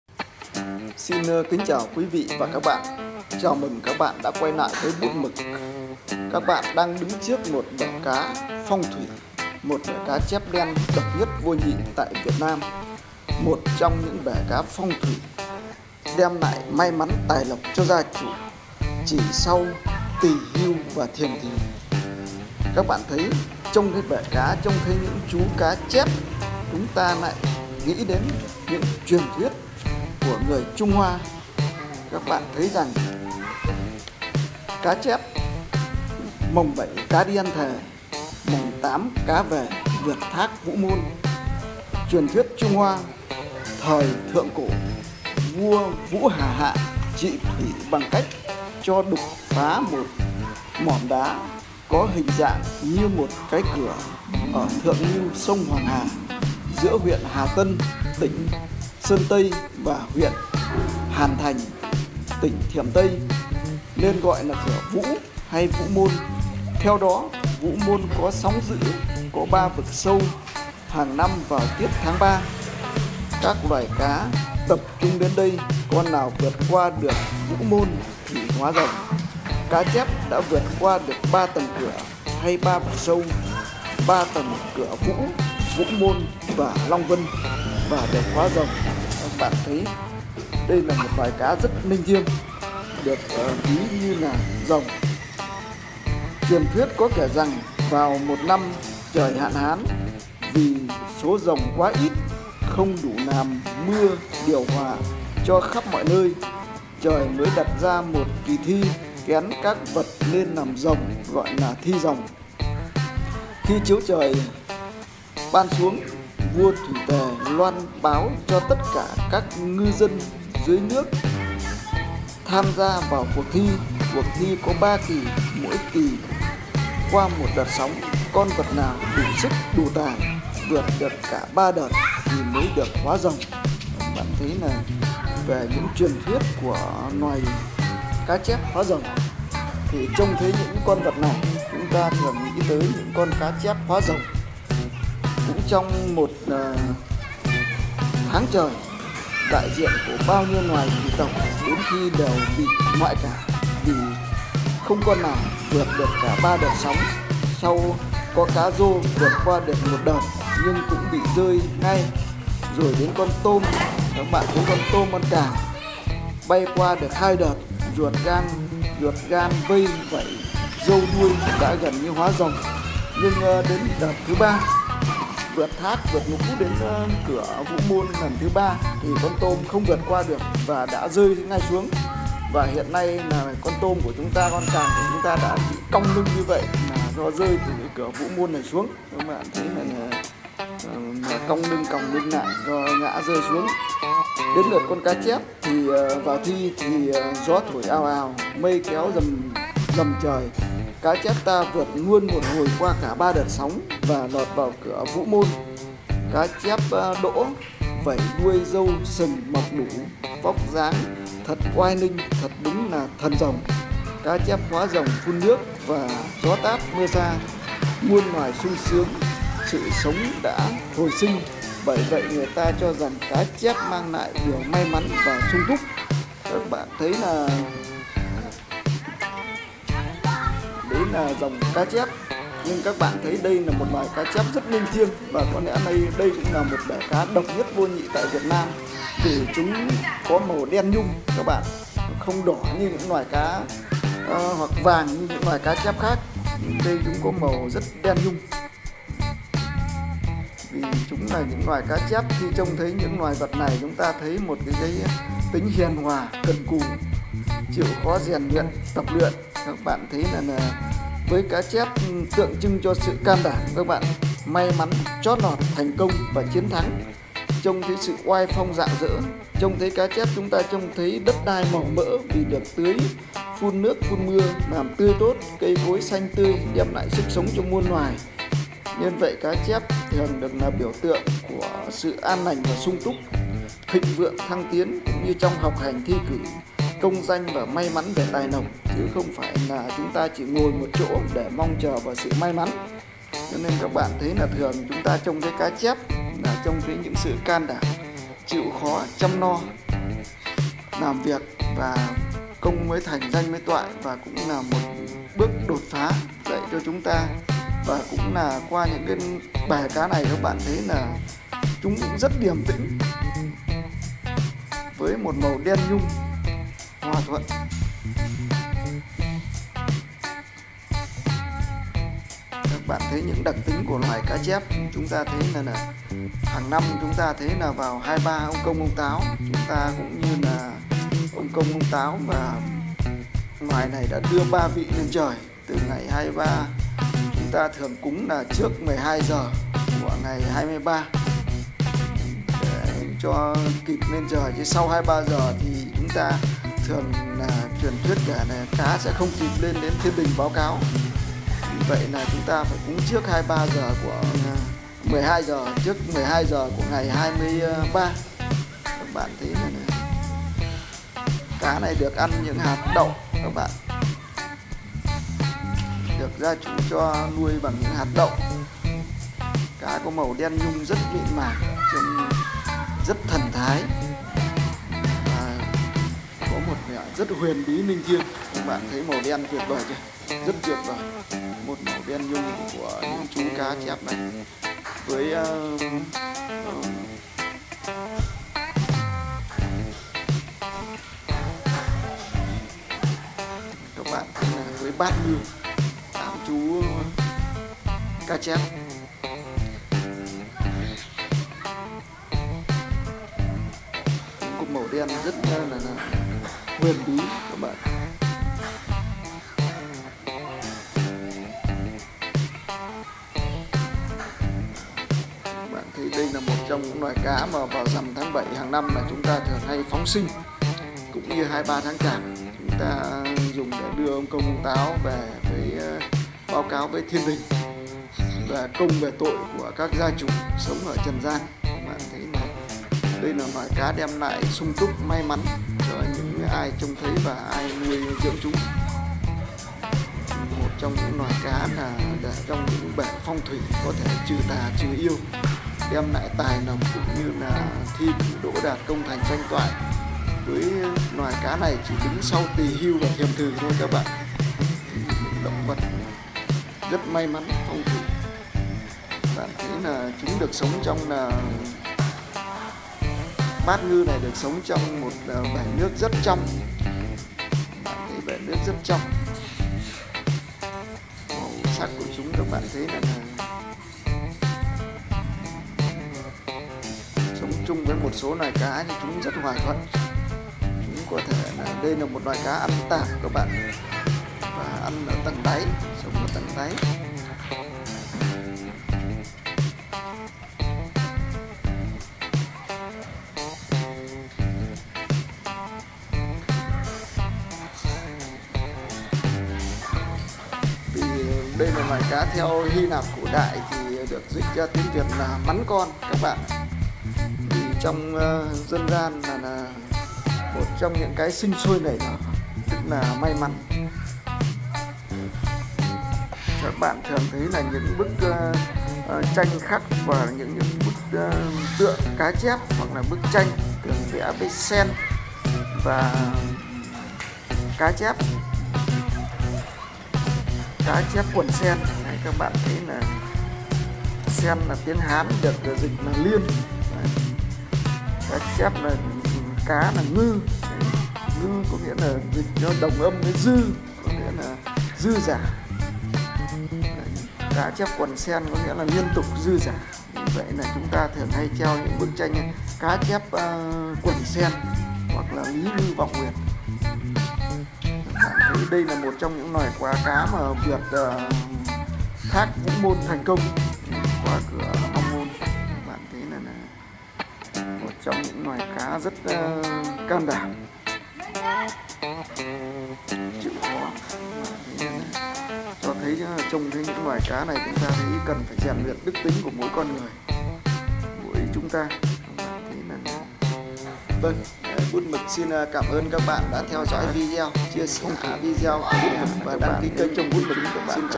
Cá chép đen
Chiêm ngưỡng những chú cá chép đen độc nhất vô nhị và tìm hiểu phong thủy về cá chép...